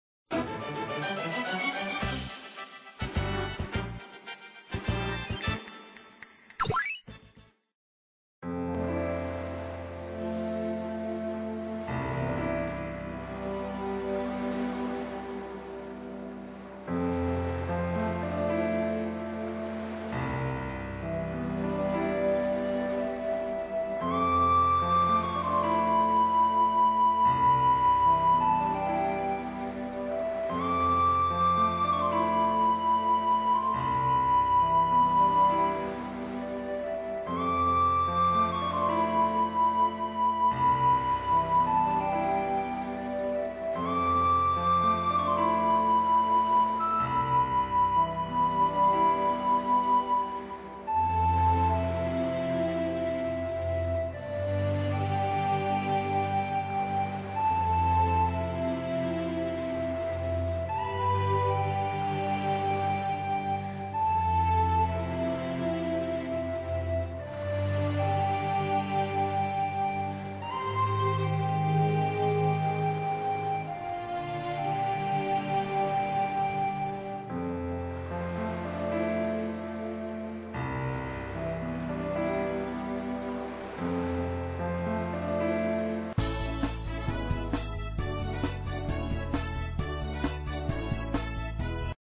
Legend of a Dude Intro [Version 2]
This is version 2 for the intro of Legend of a Dude. (Its short, but effective...I think) Advice is appreciated =D